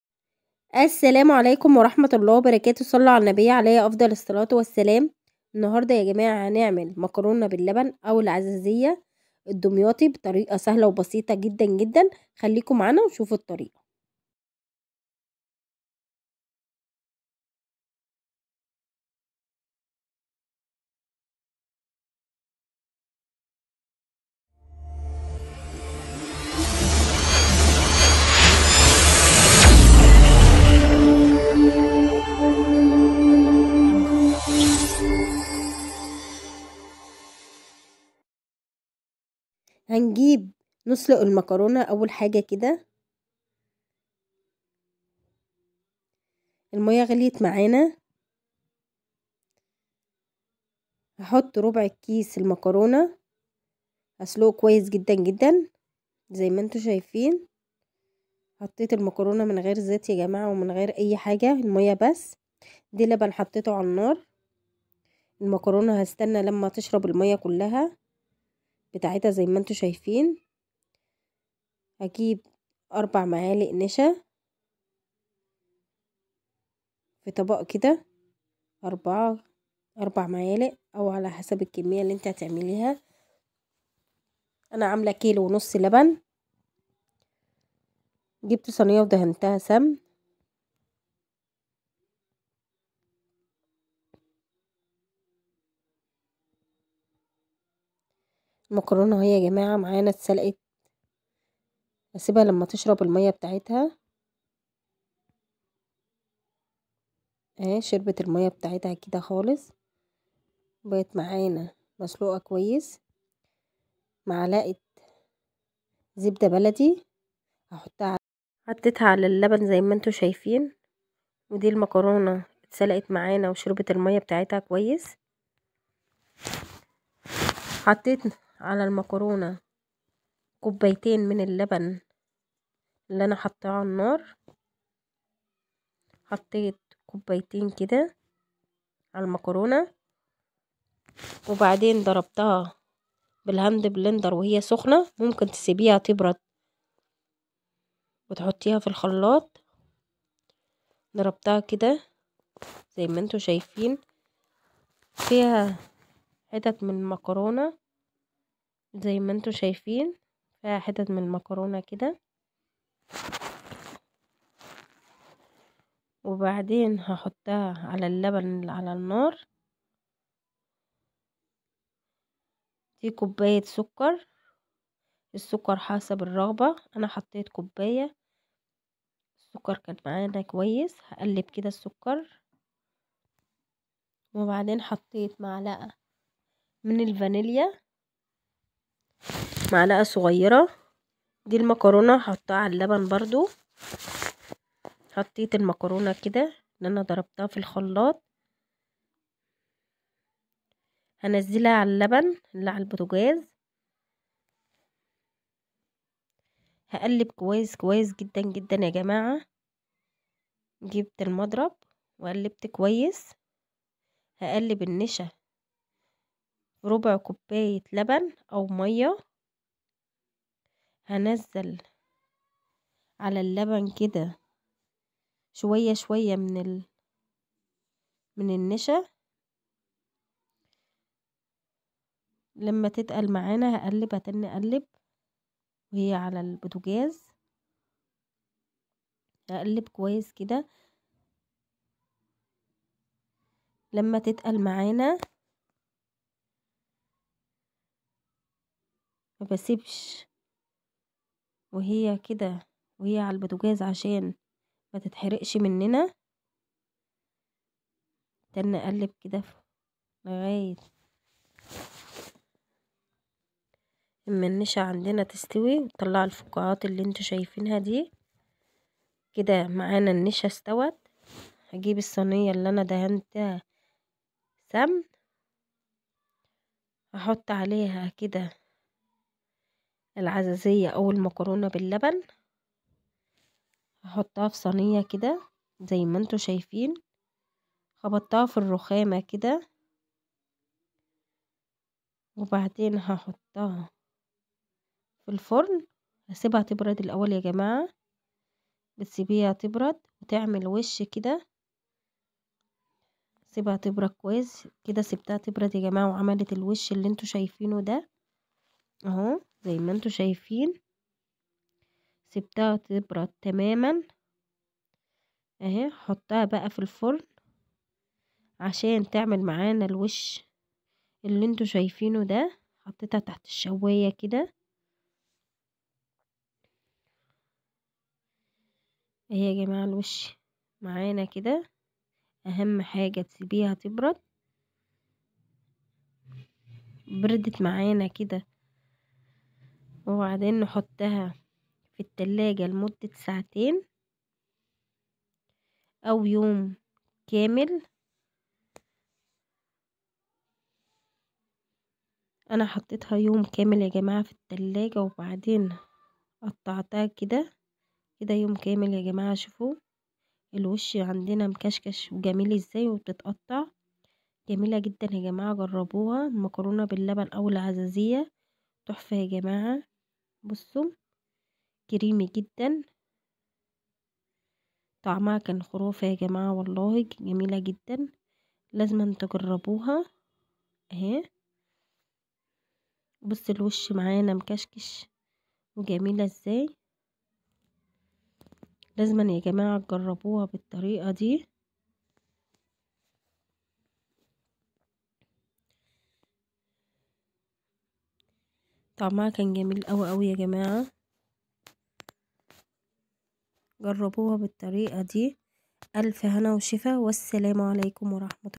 طريقه عمل العزيزيه أو مكرونه بلبن اشهر الحلويات دمياطي بطريقة أصليه ومقادير مظبوطة ونجاحه ١٠٠٪ .
مقادير العزازيه "
مكرونه _كيلو لبن ونصف _زبدة بلدي
سكر_كوب ما___________